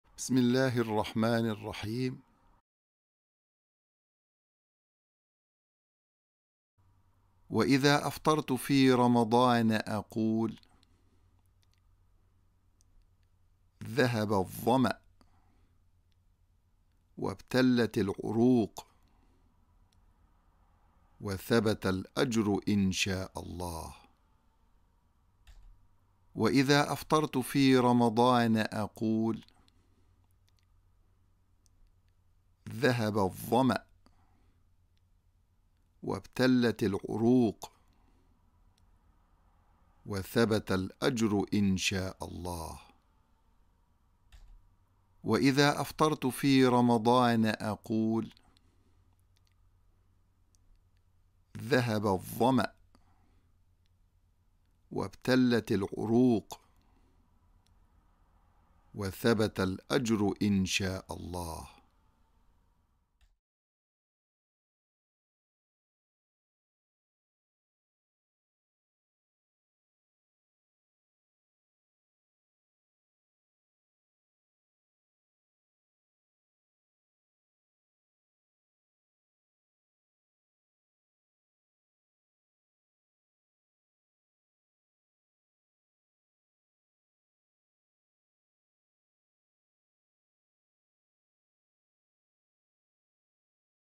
Dua - When I break my fast in ramadan
Dua for TAM Madrasat - Repeat few times with looking then write without looking. With all blessings.